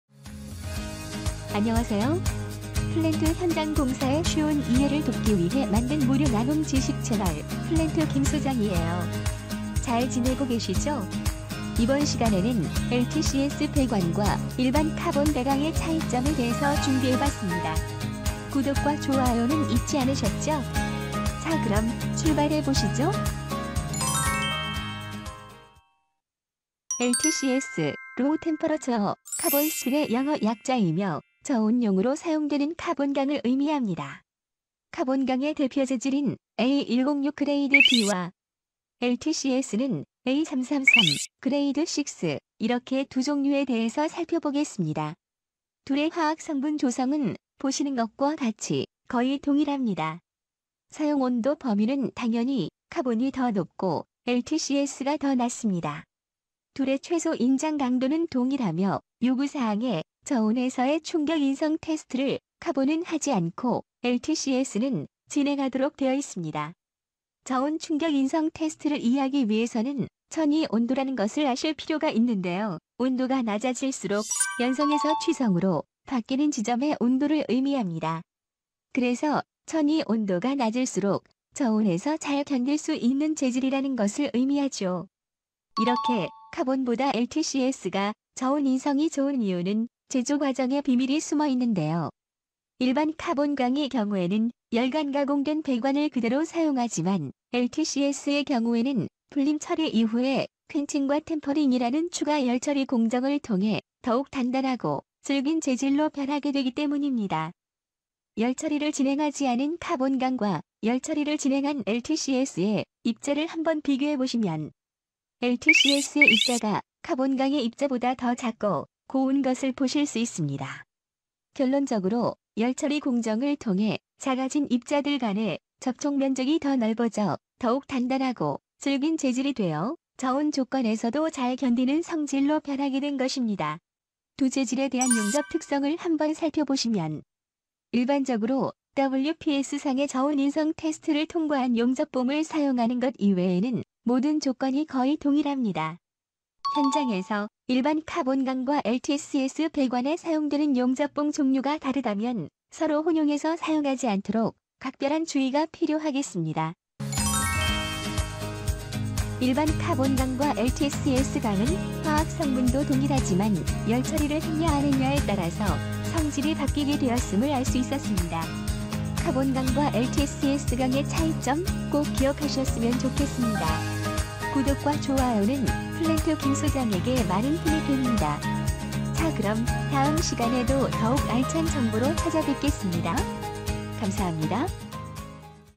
🔥[플랜트교육] LTCS vs CS 용접 차이 완벽 정리! 실전 꿀팁까지 3분 컷
👷‍♂️ 배관 설계·시공 담당자

🧪 LTCS 적용 조건 검토 중인 설계엔지니어

📘 기능사·기술사 시험 준비 중인 수험생

🛠️ 플랜트 시공, 감리, 검사 실무자

🔧 배관 자재로 자주 사용하는 LTCS와 CS! 실제로 어떤 차이가 있을까요?
용접 특성, 적용 기준, 실무에서 혼용되는 상황까지 한 번에 정리해드립니다.

🧱 LTCS vs CS 핵심 차이
**CS(Carbon Steel)**와 **LTCS(Low Temperature Carbon Steel)**는 화학 성분 자체는 거의 동일합니다.

차이는 열처리(노멀라이징) 여부에 있습니다. LTCS는 노멀라이징 처리된 CS로, 저온에서 인성(Brittle fracture) 대비가 가능한 특성을 가집니다.

LTCS는 영하 29°C 이하의 설계온도에서 적용되며, ASME VIII Div.1 Chart USC-66 또는 ASME B31.3 Fig. 323.2.2A를 기준으로 두께에 따른 저온 적용 가능 여부를 판단합니다.

일반적인 CS는 일정 두께 이상이 되면, 저온에서 깨지기 쉬운 취성 파괴 위험이 있으므로 사용에 제한이 있습니다.

💥 용접 시 주의사항
LTCS는 **충격시험(Impact Test)**이 필수인 경우가 많으며, 충격시험을 하지 않을 경우엔 설계온도 적용이 제한됩니다.

후도(두께)가 두꺼운 CS일수록 충격시험 없이 저온 적용이 어려움 → LTCS로 대체 필요

용접 시 LTCS는 강도는 높지만, 균열 가능성도 있기 때문에 **프리히팅 및 후열처리(PWHT)**에 신중을 기해야 합니다.

🚨 자재 혼용 시 대처법
🔎 CS와 LTCS가 현장에서 혼용될 위험이 있는 이유

도색 및 마킹 누락

공동 보관소, 표기 불량

스케줄 압박에 의한 식별 생략

🔧 이럴 땐 어떻게 해야 하나요?

ASME B31.3에 따라 Stress Ratio 개념으로 저온 설계온도 보정이 가능합니다.

Stress Ratio 계산을 통해 필요한 경우, 추가 지지대 설치로 응력 완화

Stress Ratio ≤ 0.65일 경우, 설계온도 하향(-17°C 이상) 적용도 가능

📌 추가 정보
갈바나이징 된 CS는 부식 저항은 뛰어나지만, 용접 시 아연층 제거 필요

용접 후 도장 보완이 필요하고, 고온 배관이나 위생/청정도 요구 배관에는 사용 부적합